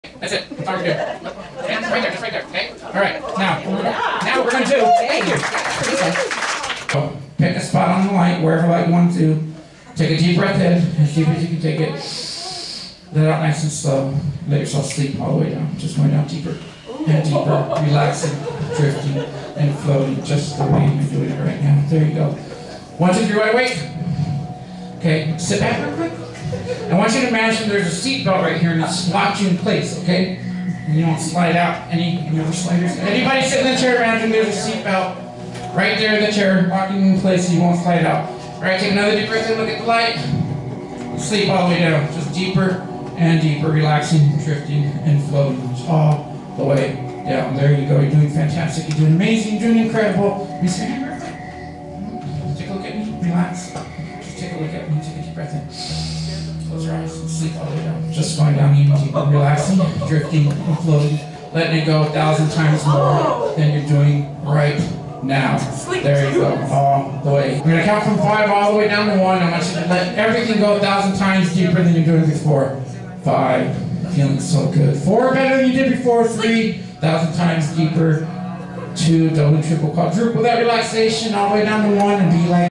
Did you know children respond exceptionally well to hypnosis???
Accompanied the amazing BeeHypnotized (Ben Garcia) for a family stage hypnosis show at the The Sierra Vista Community Theatre in Sierra Vista, AZ.

DID YOU KNOW HYPNOSIS WORKS VIRTUALLY?!